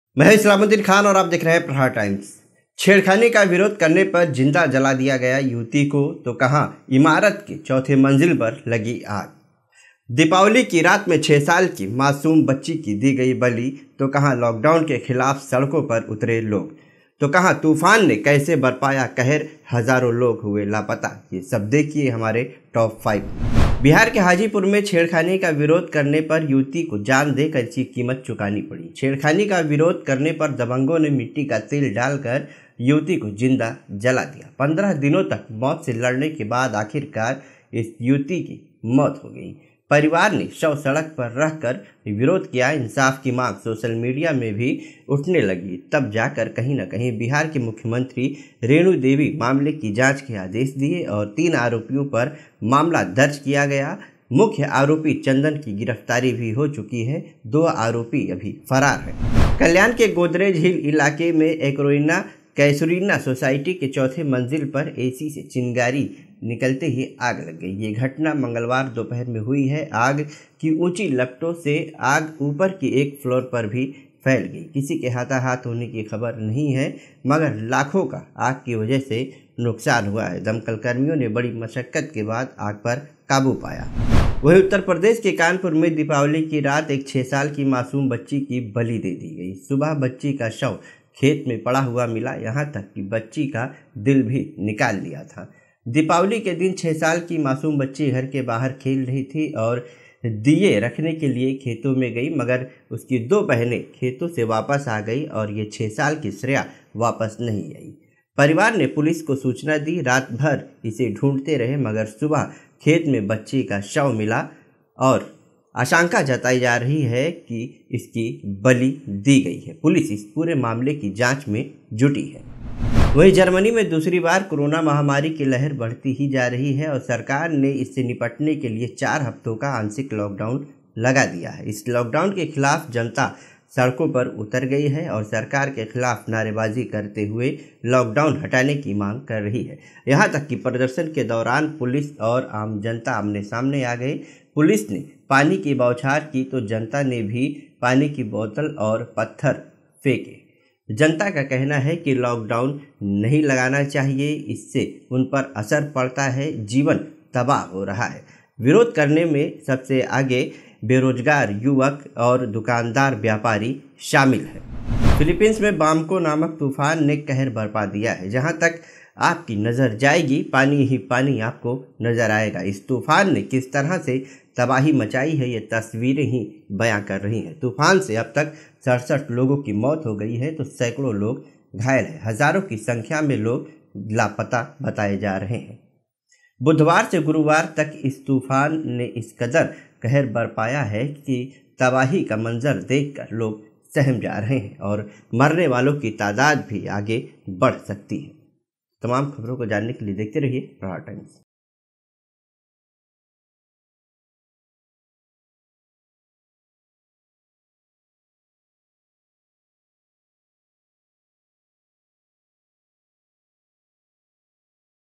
लड़की को जिंदा जलाया, बच्ची की बलि, लॉकडाउन के खिलाफ सड़क पर उतरे लोग || [ TOP 5 ] Aapka Prahar Times
लड़की को जिंदा जलाया, बच्ची की बलि, लॉकडाउन के खिलाफ सड़क पर उतरे लोग